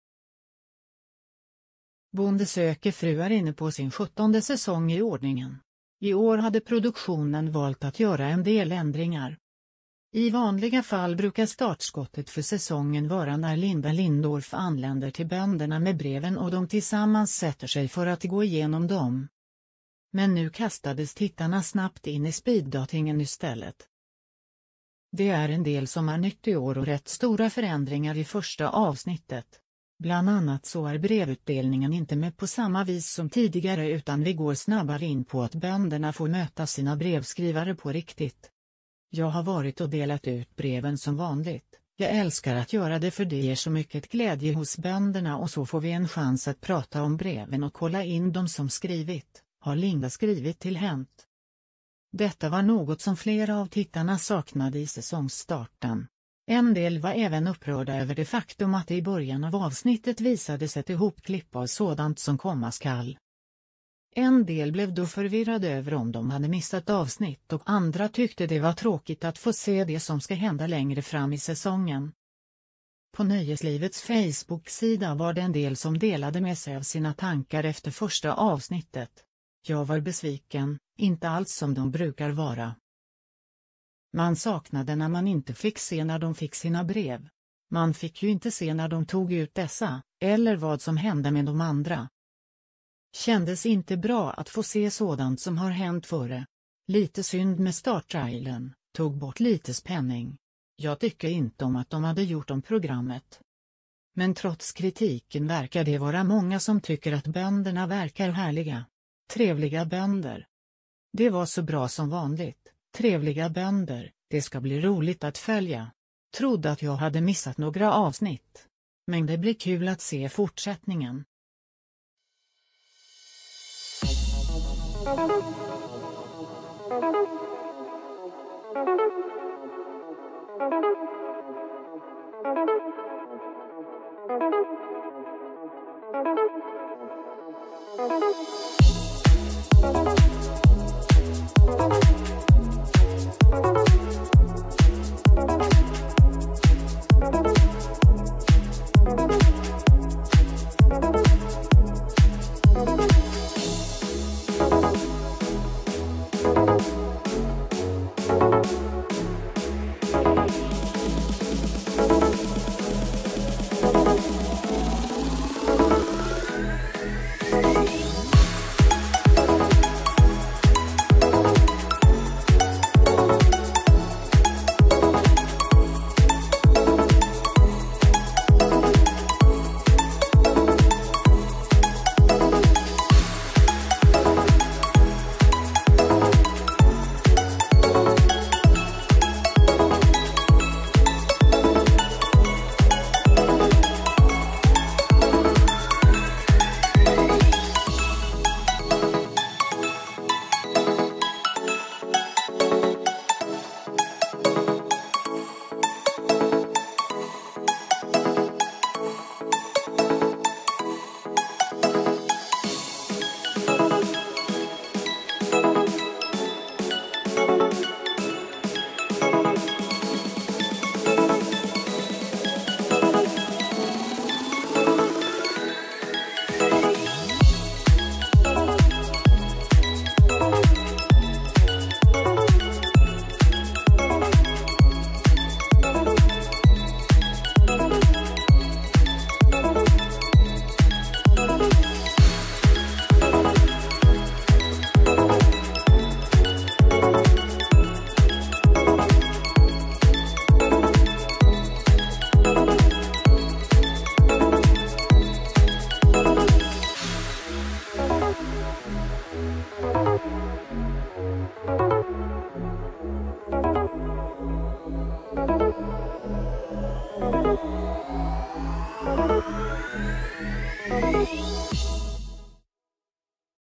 Tittarnas vrede mot Bonde söker fru – efter oväntade händelsen i TV4 ”Kändes inte bra”
En ny säsong av det populära programmet ”Bonde söker fru” har gått av stapeln.

Under onsdagskvällen satt många bänkade för att se det första avsnittet för i år.

Men nu riktar tittarna kritik mot den nya säsongen.

– Jag var besviken, skriver en av tittarna på Nöjeslivets Facebooksida.